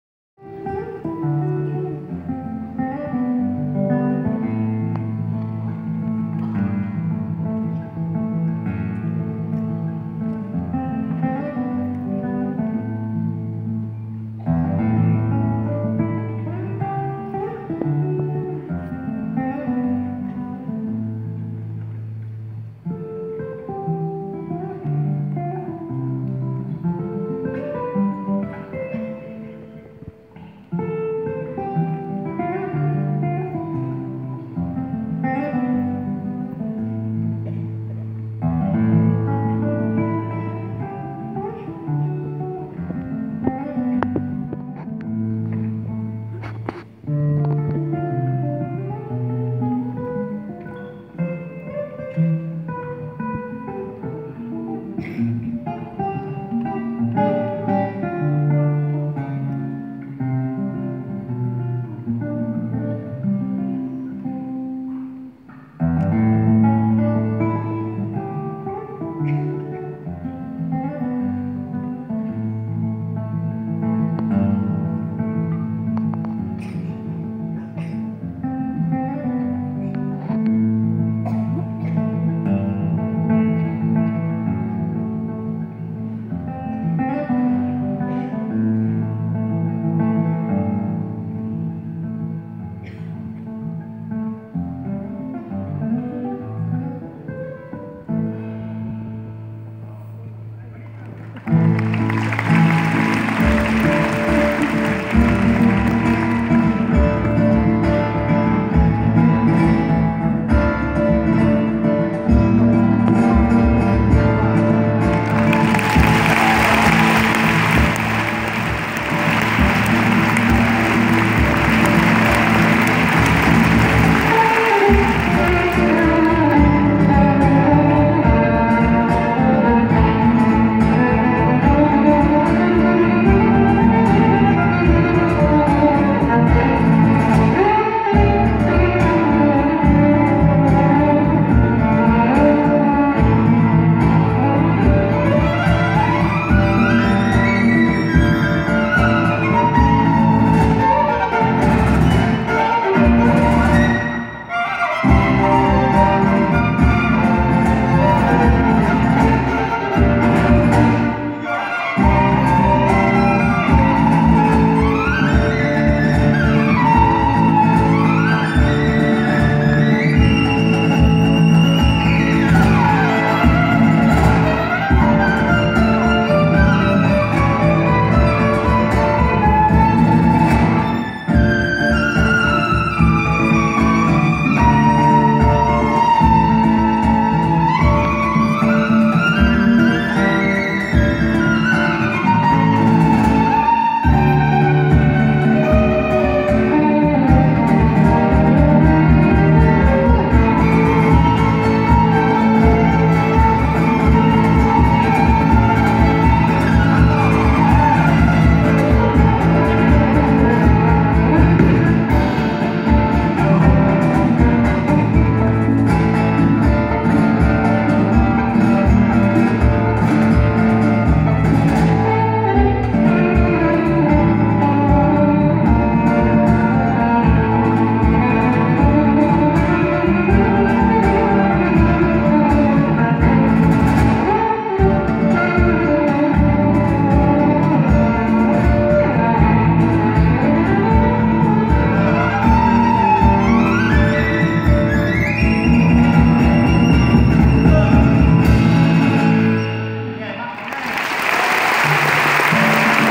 Vasko Vassilev ~ Passion Flamenca tour 2019 ~ Live 1
Vasko Vassilev ~ Passion Flamenca tour 2019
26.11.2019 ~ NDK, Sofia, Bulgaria
Vasko Vassilev, violin
Victor Vallejo, keyboard / trumpet
Juan Heredia, percussions
David Buckingham, guitar